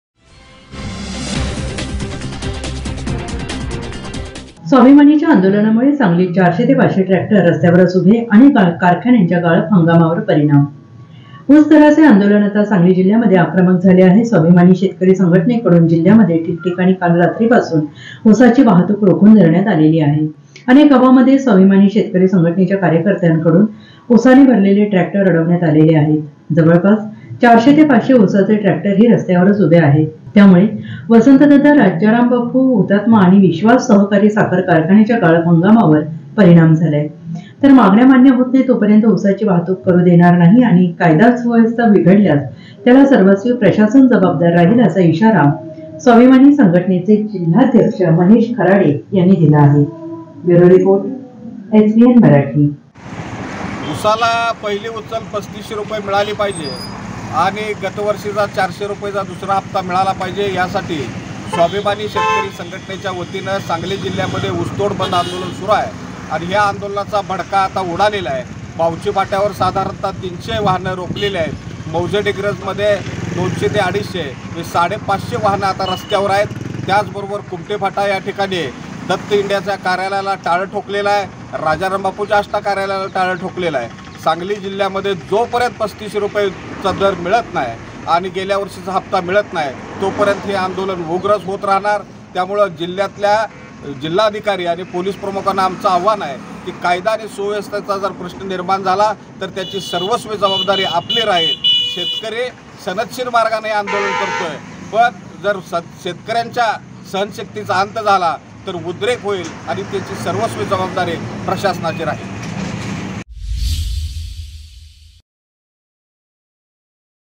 SANGLI | स्वाभिमानीच्या आंदोलनामुळे सांगलीत 400 ते 500 ट्रॅक्टर रस्त्यावरच उभे!
स्वाभिमानीच्या आंदोलनामुळे सांगलीत 400 ते 500 ट्रॅक्टर रस्त्यावरच उभे! अनेक कारखान्याच्या गाळप हंगामावर परिणाम.